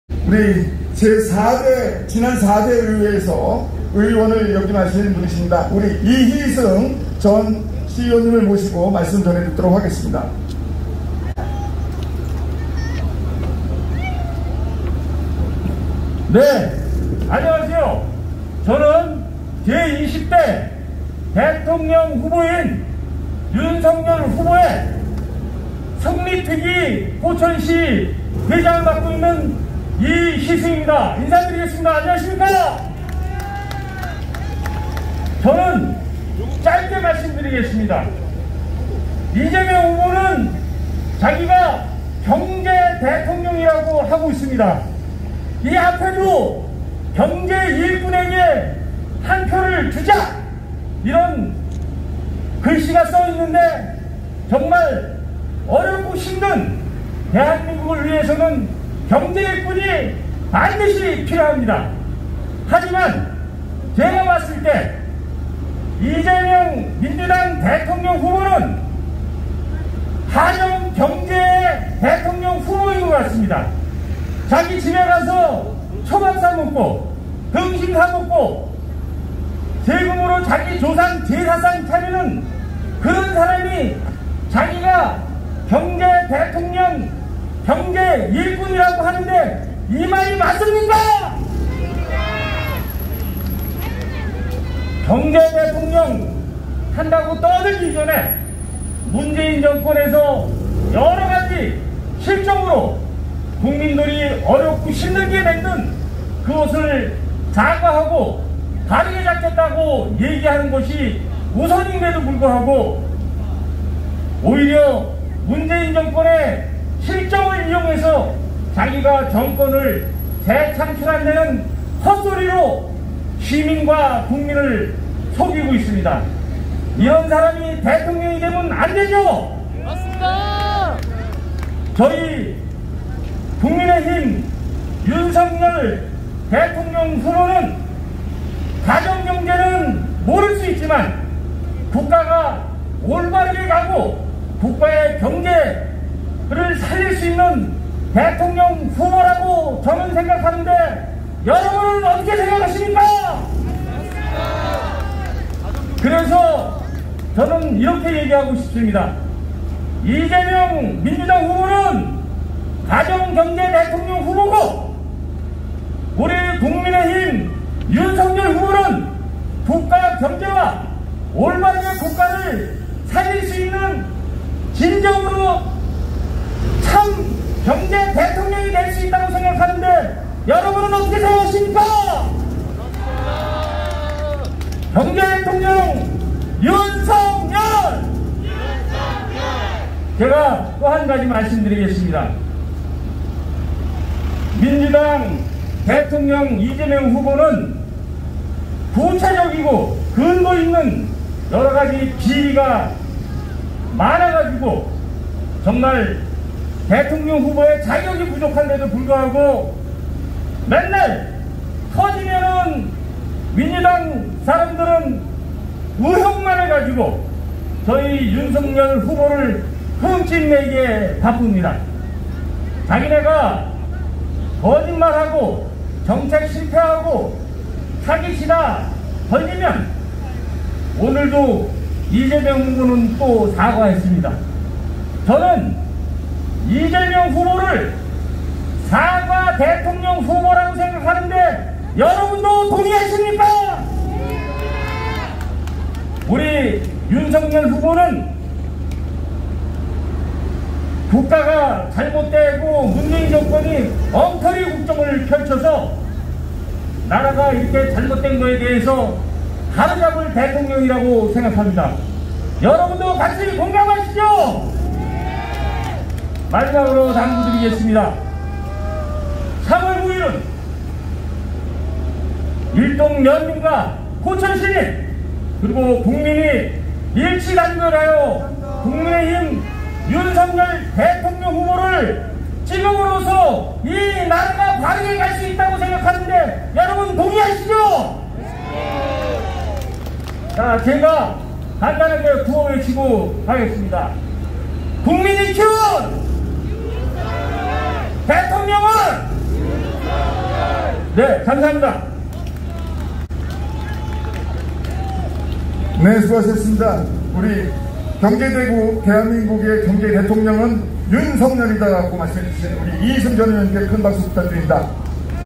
제20대 대통령선거/윤석열 대통령후보/지원유세/이희승 연사/일동면/직캠
윤석열대통령후보 지원유세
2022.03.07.